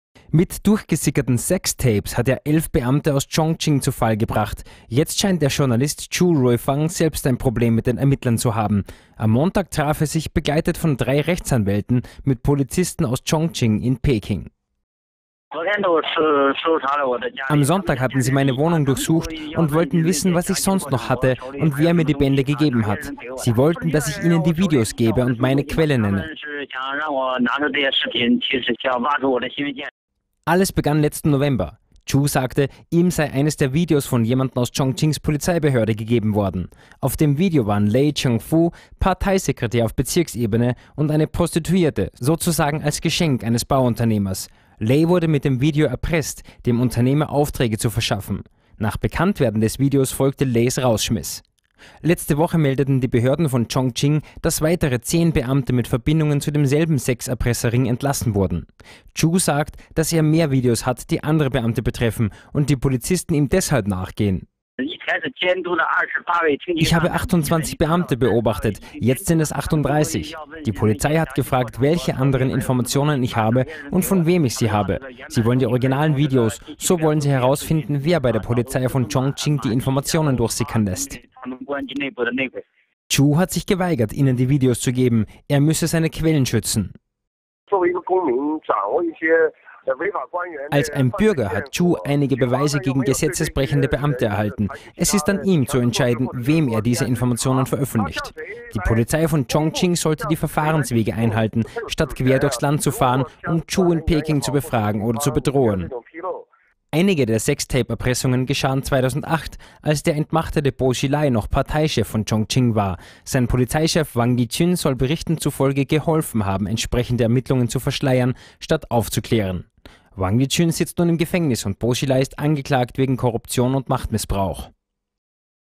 Sextapes belasten Beamte von Chongqing - Polizei sucht Quelle von Journalistem
Mit durchgesickerten Sextapes hat er elf Beamte aus Chongqing zu Fall gebracht. Jetzt scheint der Journalist Zhu Ruifeng selbst ein Problem mit den Ermittlern zu haben. Am Montag traf er sich, begleitet von drei Rechtsanwälten, mit Polizisten aus Chongqing in Peking.

[Zhu Ruifeng, Journalist]:
„Am Sonntag hatten sie meine Wohnung durchsucht und wollten wissen, was ich sonst noch hatte und wer mir die Bänder gegeben hat. Sie wollten, dass ich ihnen die Videos gebe und meine Quelle nenne."

Alles begann letzten November. Zhu sagte, ihm sei eines der Videos von jemandem aus Chongqings Polizeibehörde gegeben worden. Auf dem Video waren Lei Zhengfu, Parteisekretär auf Bezirksebene, und eine Prostituierte, sozusagen als „Geschenk" eines Bauunternehmers. Lei wurde mit dem Video erpresst, dem Unternehmer Aufträge zu verschaffen. Nach Bekanntwerden des Videos folgte Leis Rausschmiss.

Letzte Woche meldeten die Behörden von Chongqing, dass weitere zehn Beamte mit Verbindungen zu demselben Sexerpresserring entlassen wurden.

Zhu sagt, dass er mehr Videos hat, die andere Beamte betreffen, und die Polizisten ihm deshalb nachgehen.

[Zhu Ruifeng, Journalist]:
„Ich habe 28 Beamte beobachtet, jetzt sind es 38. Die Polizei hat gefragt, welche anderen Informationen ich habe und von wem ich sie habe. Sie wollen die originalen Videos, so wollen sie herausfinden, wer bei der Polizei von Chongqing die Informationen durchsickern lässt."

Zhu hat sich geweigert, ihnen die Videos zu geben. Er müsse seine Quellen schützen.

[Tang Jitian, Rechtsanwalt, Peking]:
„Als ein Bürger hat Zhu einige Beweise gegen gesetzesbrechende Beamte erhalten. Es ist an ihm, zu entscheiden, wem er diese Informationen veröffentlicht. Die Polizei von Chongqing sollte die Verfahrenswege einhalten, statt quer durchs Land zu fahren, um Zhu in Peking zu befragen oder zu bedrohen."
Einige der Sextape-Erpressungen geschahen 2008, als der entmachtete Bo Xilai noch Parteichef von Chongqing war. Sein Polizeichef Wang Lijun soll Berichten zufolge geholfen haben, entsprechende Ermittlungen zu verschleiern, statt aufzuklären.

Wang Lijun sitzt nun im Gefängnis und Bo Xilai ist angeklagt wegen Korruption und Machtmissbrauch.